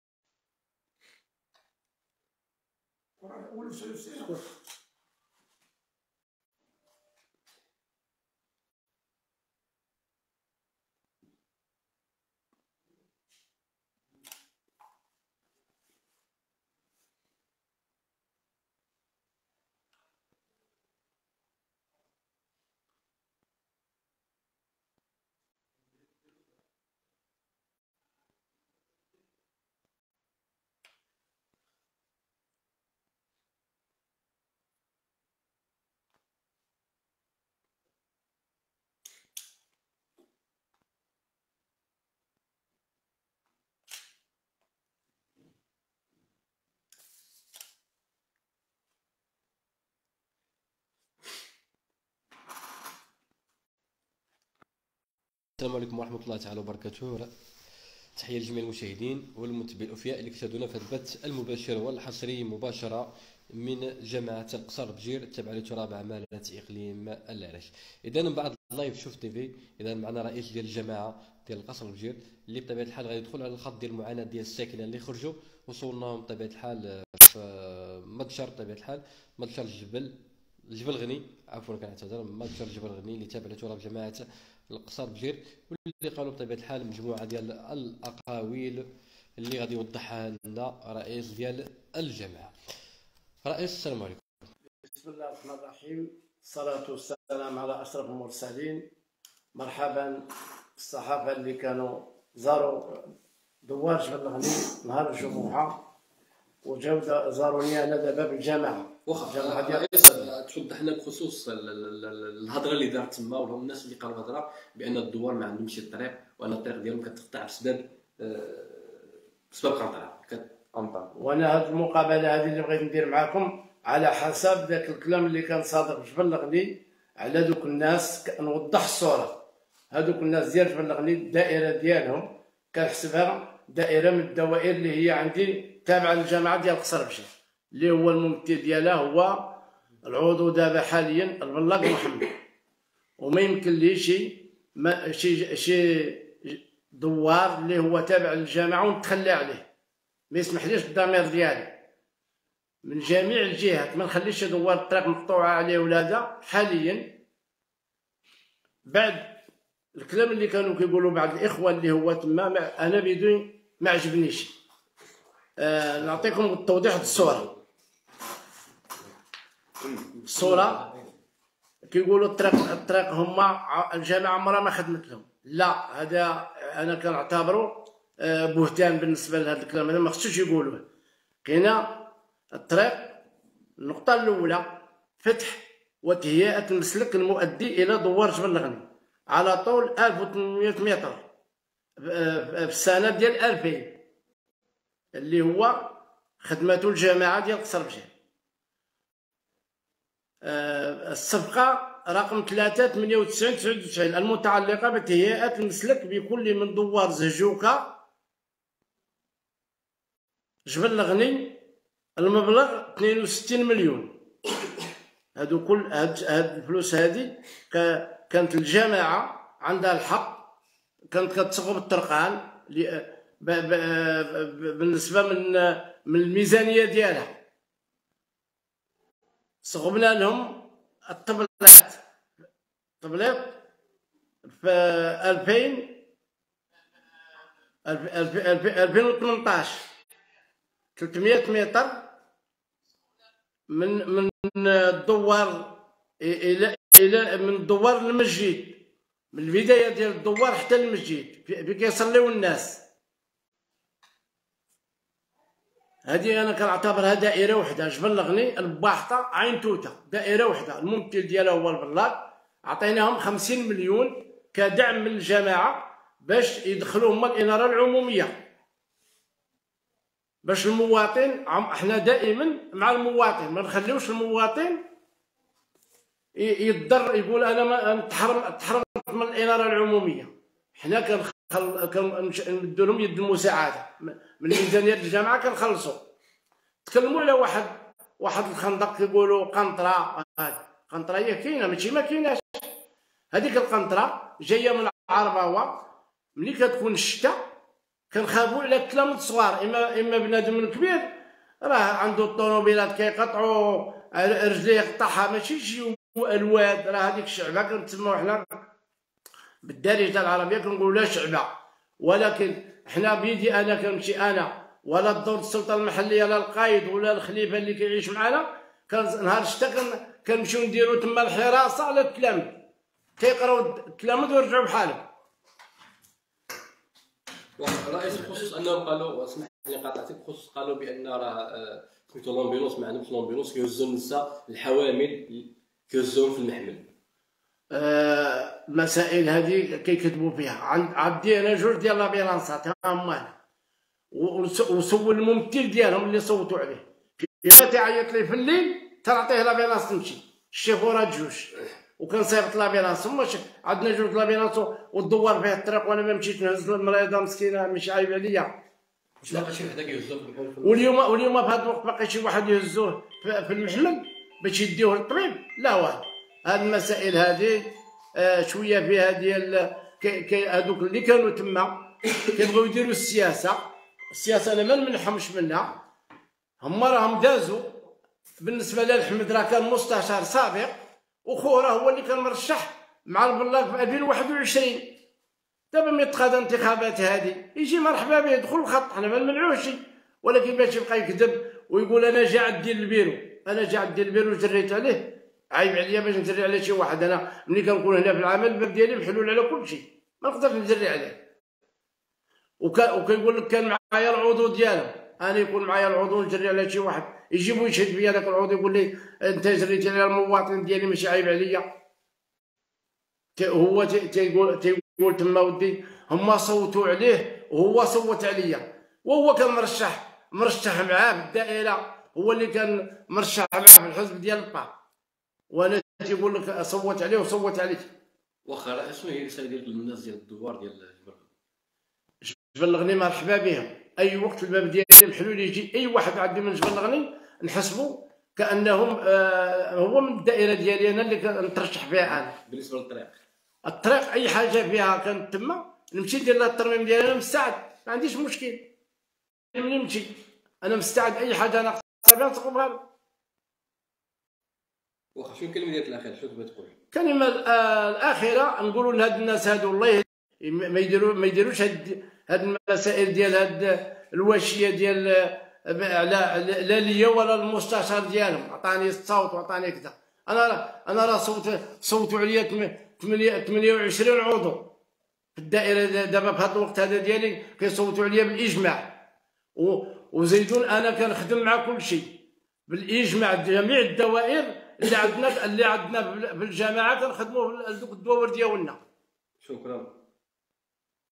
رئيس الجماعة يدخل على خط معاناة سكان المنطقة: "خصهم يصبرو علينا شوية راحنا خدامين"
مباشرة من جماعة القصر بجير إقليم العرائش: بعد لايف شوف تيفي.. رئيس الجماعة يدخل على خط معاناة سكان المنطقة: "خصهم يصبرو علينا شوية راحنا خدامين"